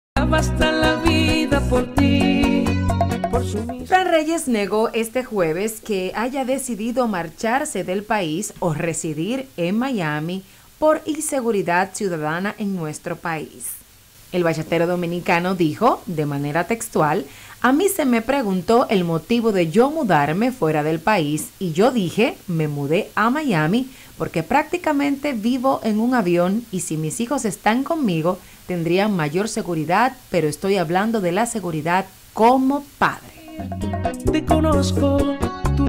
Frank Reyes niega se marchara del país por la inseguridad ciudadana
El cantante de bachata dominicano Frank Reyes negó este jueves que haya decidido marcharse de la República Dominicana por miedo a la supuesta inseguridad ciudadana como erróneamente, afirmó, se ha difundido en redes sociales.